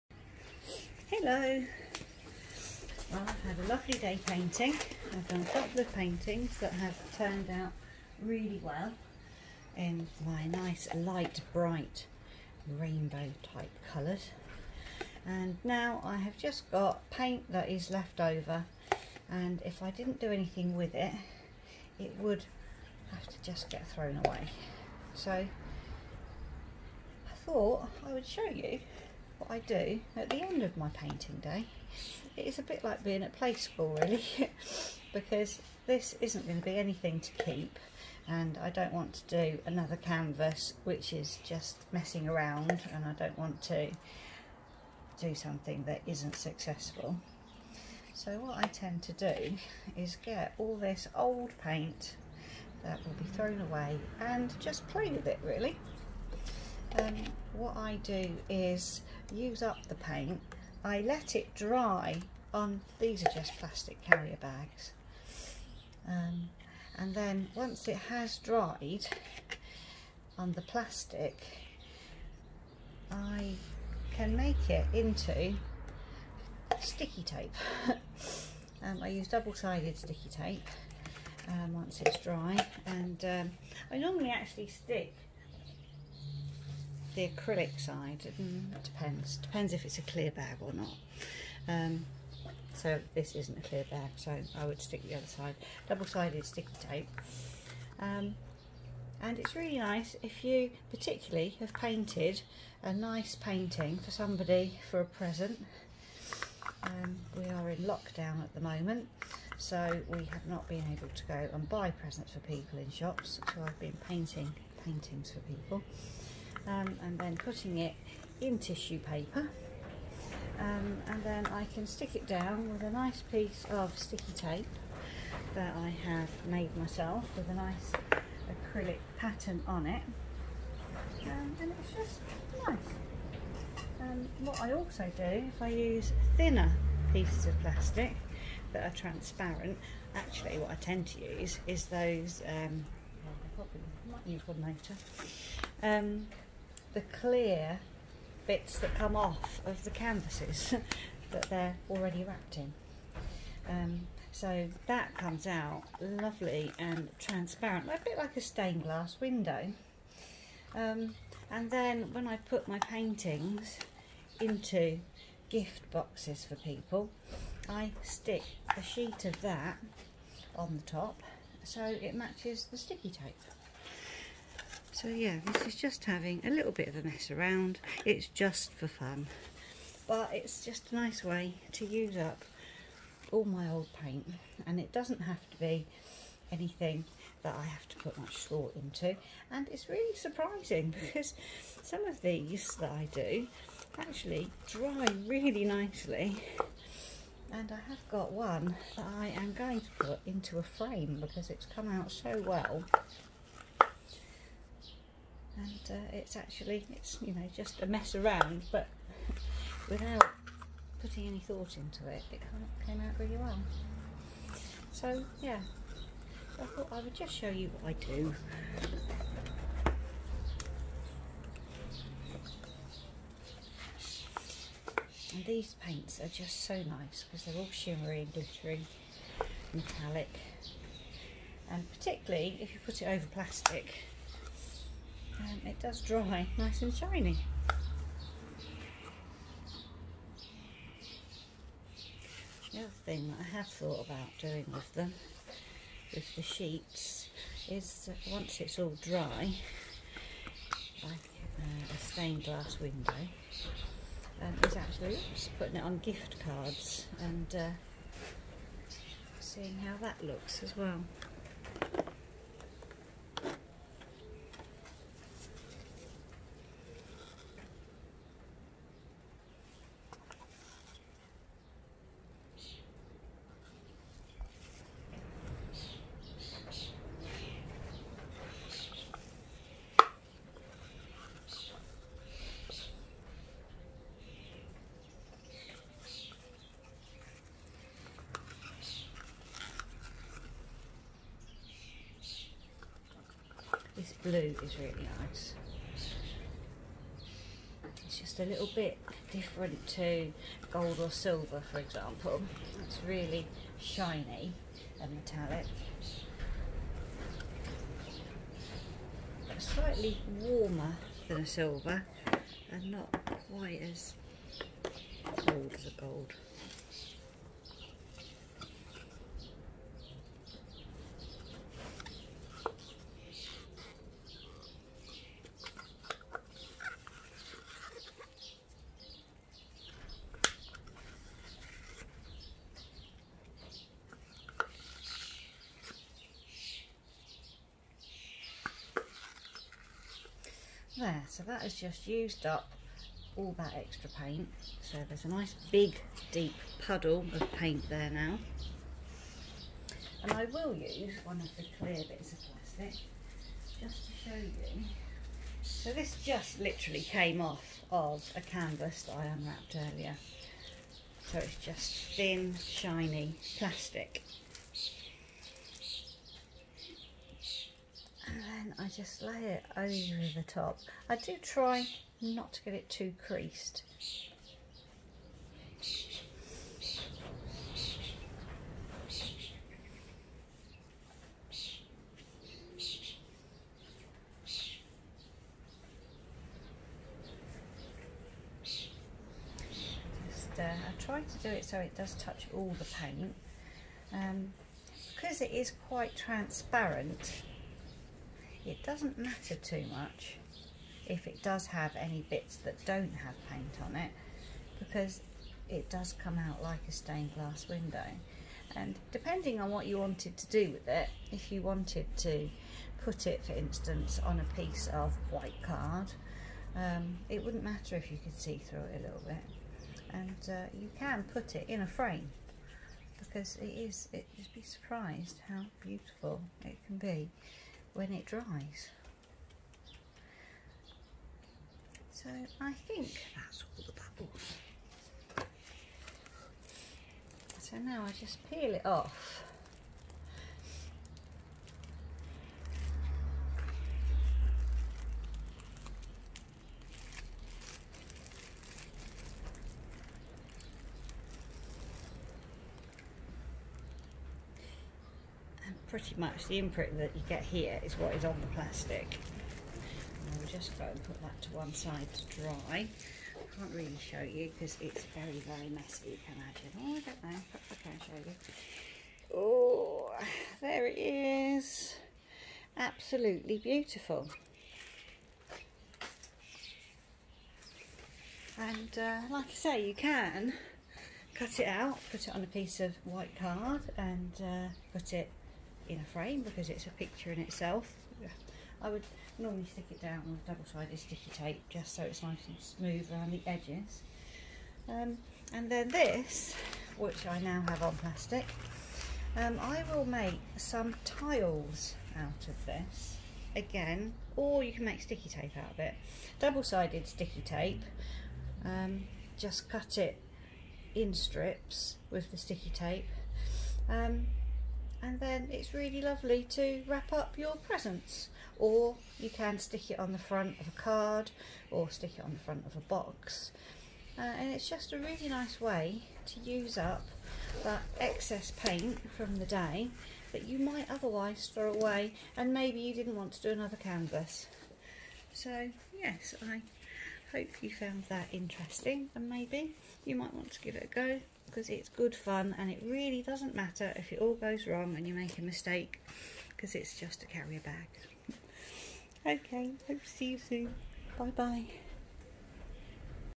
#3 Using up the left over acrylic paint from the day
I love to experiment and have fun trying new ideas!
Can be used to make gift cards, tags and home made sticky tape or even go into a little frame.